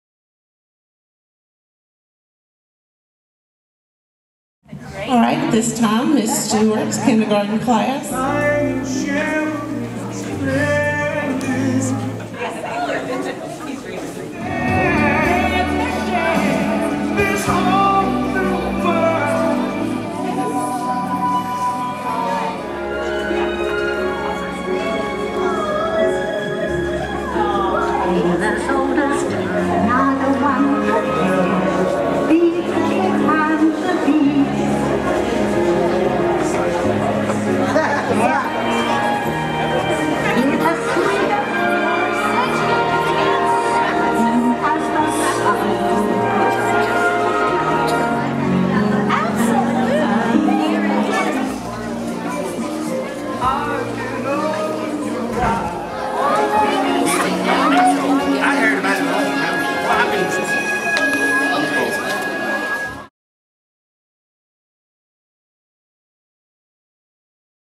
Cinderella Ball
The Annual Cinderella Ball at Gardner Park Elementary School.